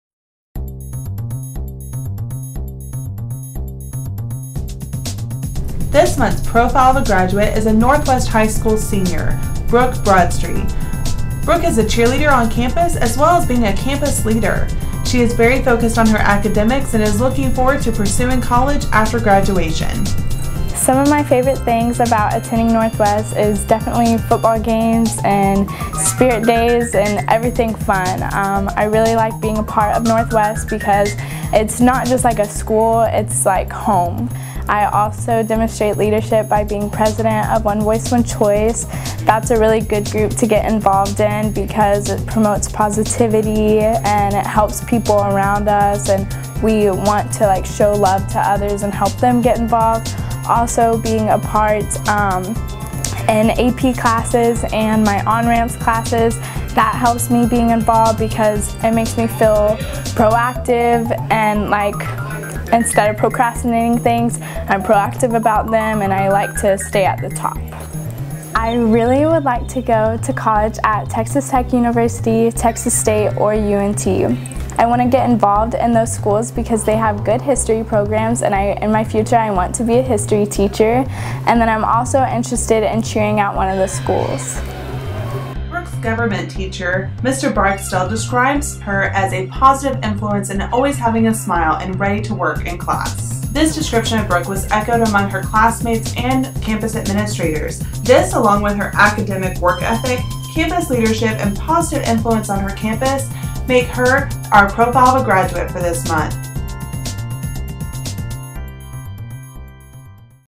Profile of a Graduate: Brooke Broadstreet
Brooke is a senior at Northwest High School. She is dedicated to her academics and is known for her positive demeanor. In the future she hopes to pursue a teaching career and continue her passion of cheerleading.